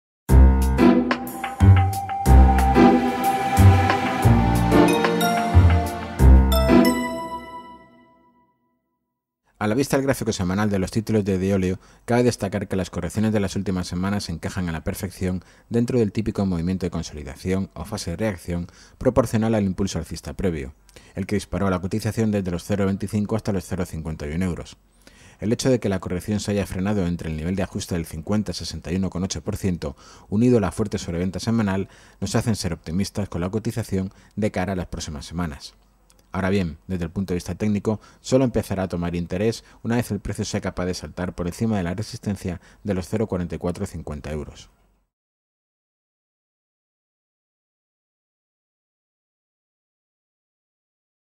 Análisis técnico de Deoleo
El aspecto semanal del gráfico no es malo. Se encuentra en "fase de reacción" y lo cierto es que parece que, más pronto que tarde, podría reestructurarse al alza. Ahora bien, desde el punto de vista técnico cabe decir que la nueva señal de compra vendrá de la mano de la superación de los 0,445 euros.